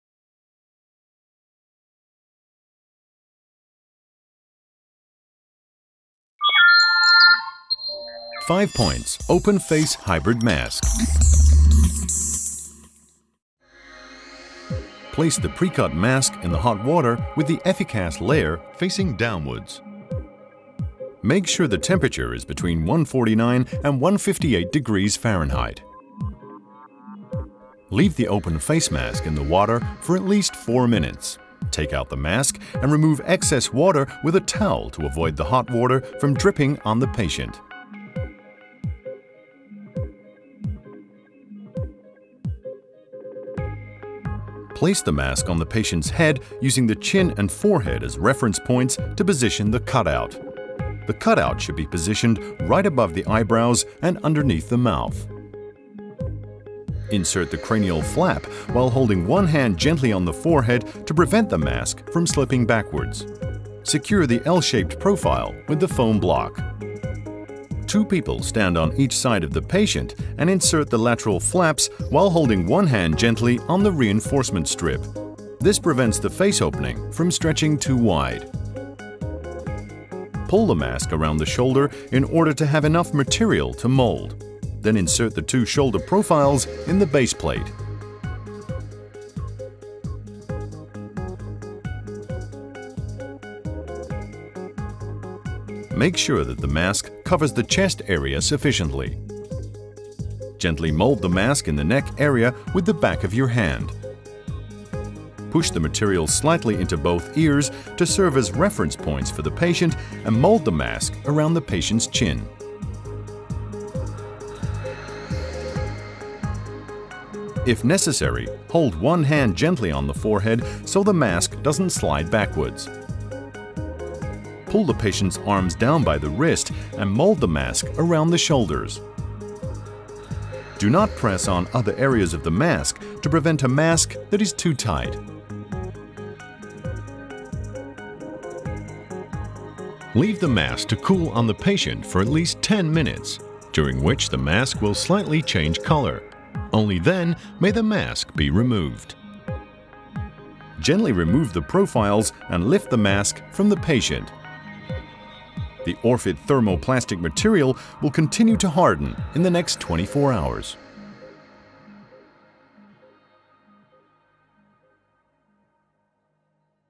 5-points Open Face Hybrid Mask: Instruction Video
The Orfit Open Face Hybrid Mask leaves eyes, nose and mouth exposed, and is, as such, a brand new device for adequate radiation therapy for the head and neck area. It warrants increased patient comfort and provides the same high precision immobilization as the standard head and neck masks.